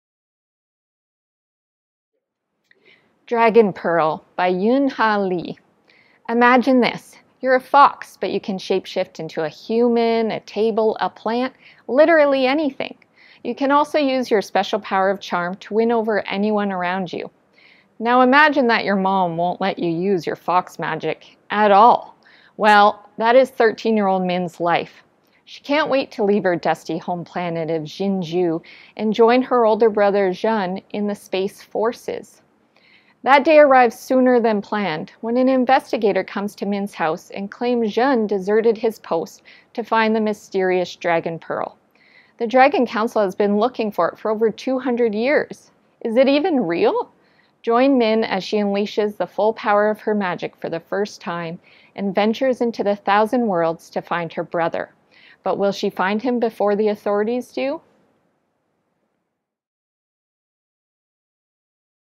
Dragon Pearl by Yoon Ha Lee | YRCA 2022 Junior Nominee presented by EPL
The Edmonton Public Library presents the 2022 YRCA Junior Nominee, Dragon Pearl by Yoon Ha Lee.

Min, a thirteen-year-old girl with fox-magic, runs away from her home planet to solve the mystery of her missing brother.